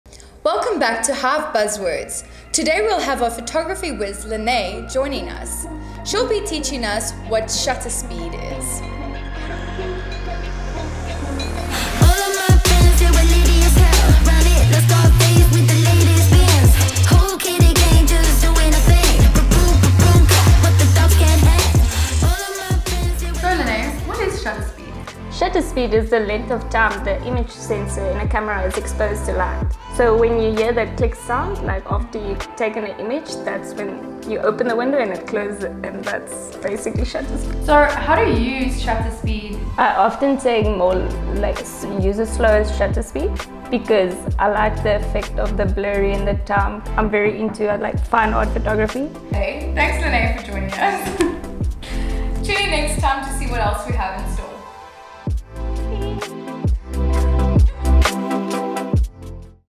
Hive Buzz Words Ep. 2 - Shutter Speed
We had Lené Swanepoel, one of our lens maestros tell us what exactly is shutter speed and why it is so important.